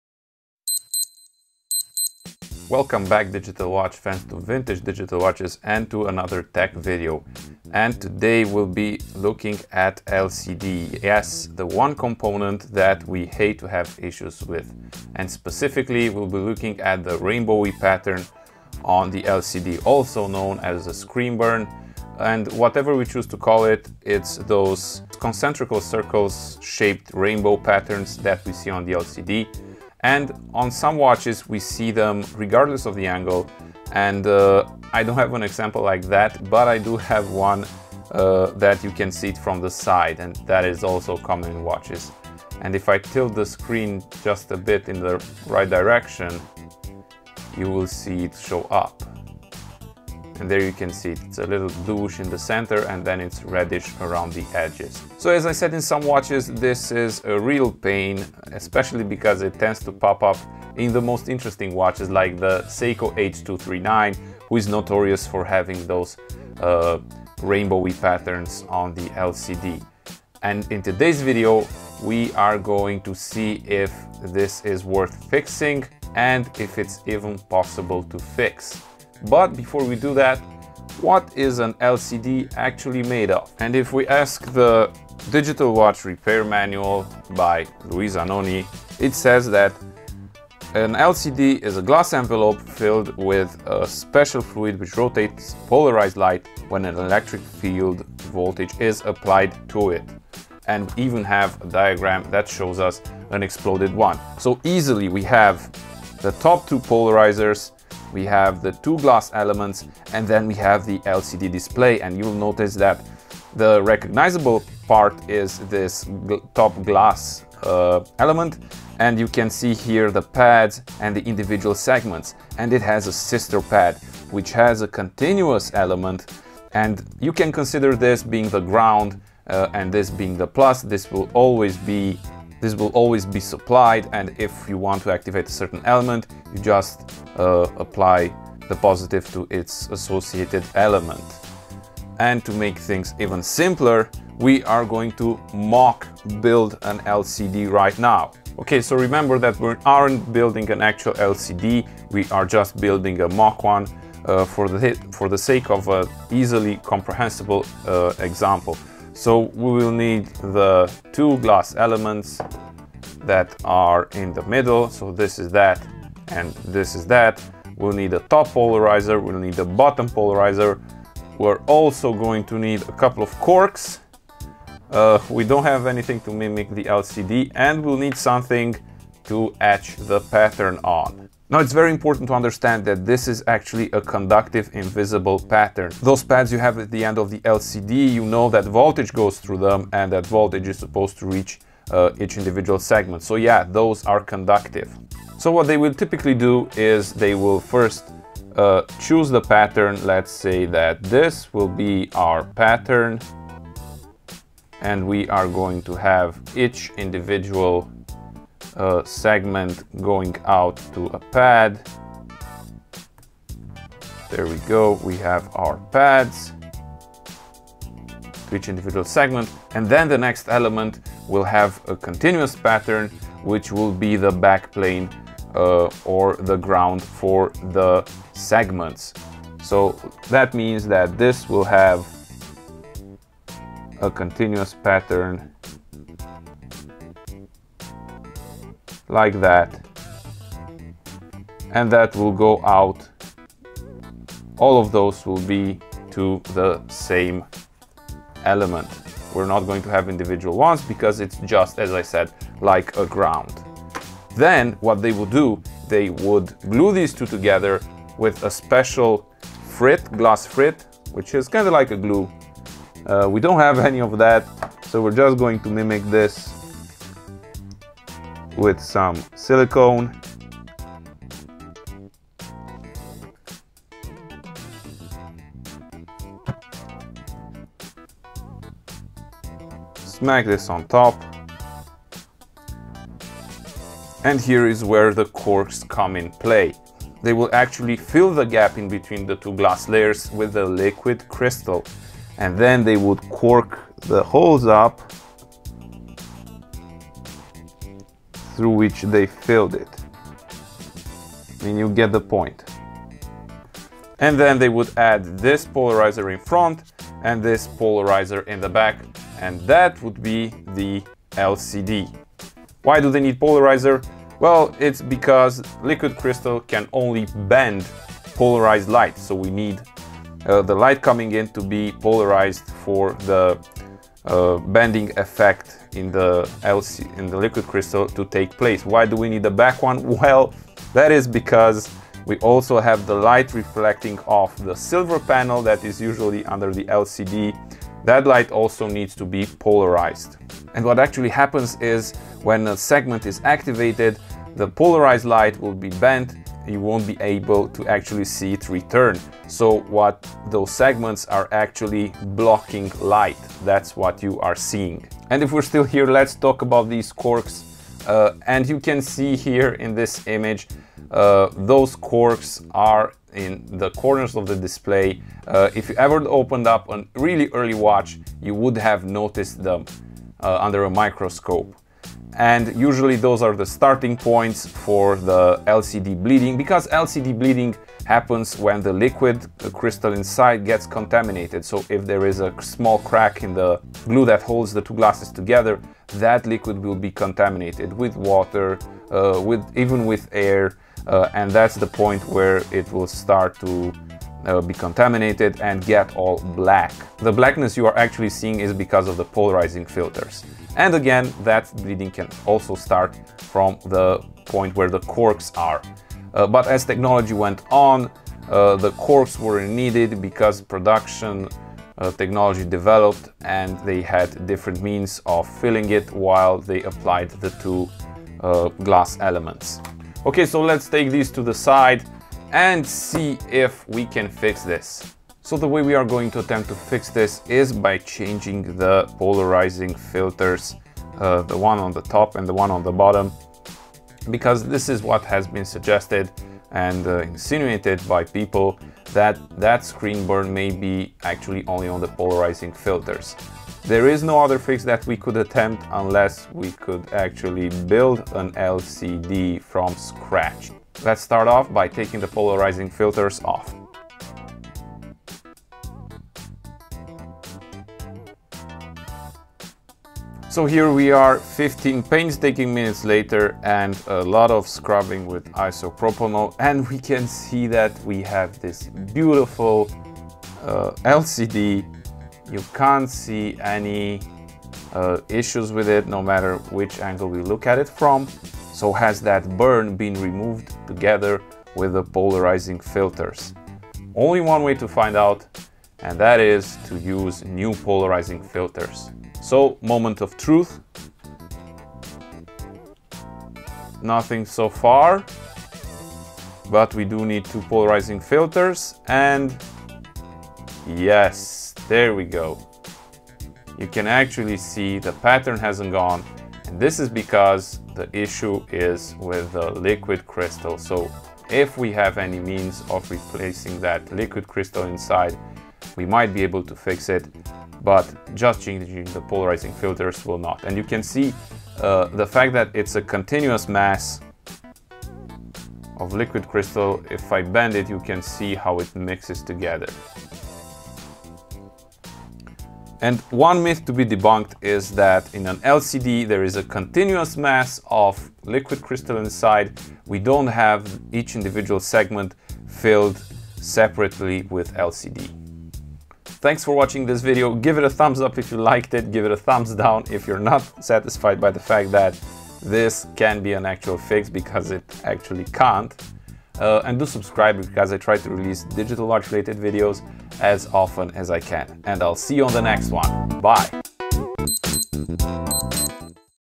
LCD burn fix attempt? - Ep 67 - Vintage Digital Watches
So I finally got around to attempt a fix for the rainbowy pattern that some LCDs have, success? or NOT?